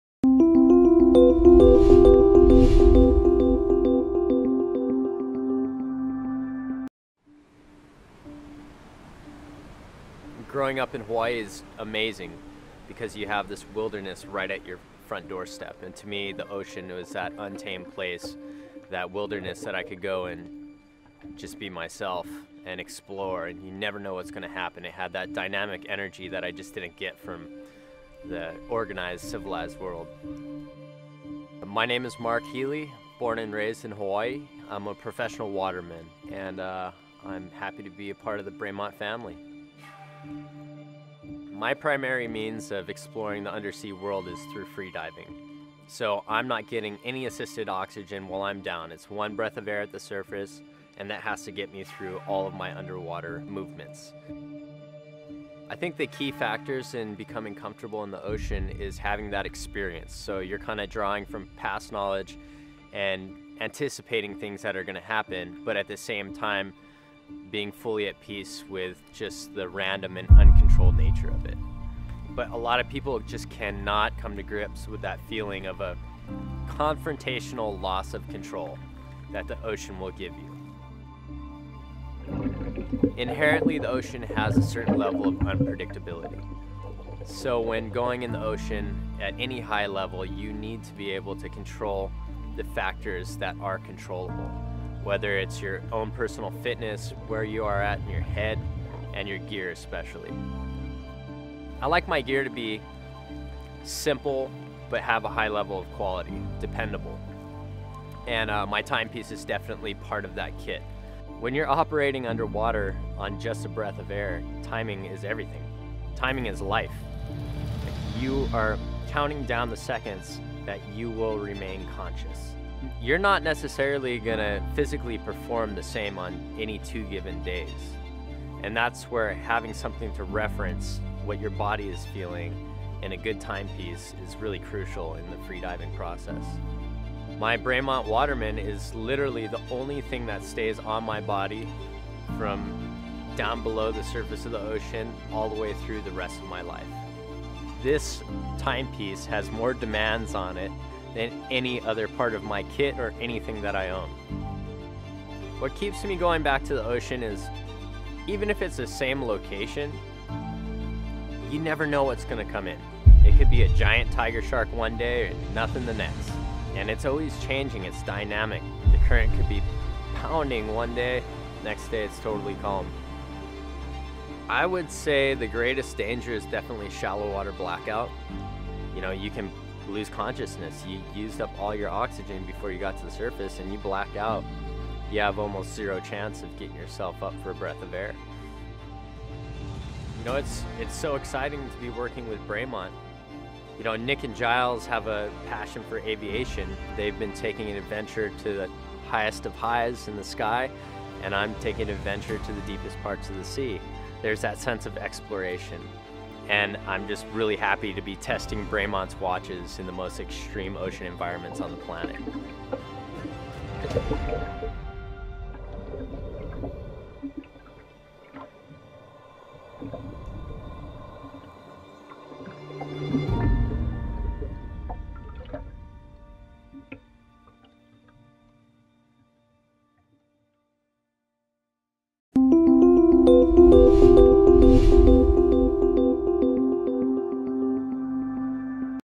The Bremont Waterman - Launch
Proudly announcing Bremont's new Waterman timepiece.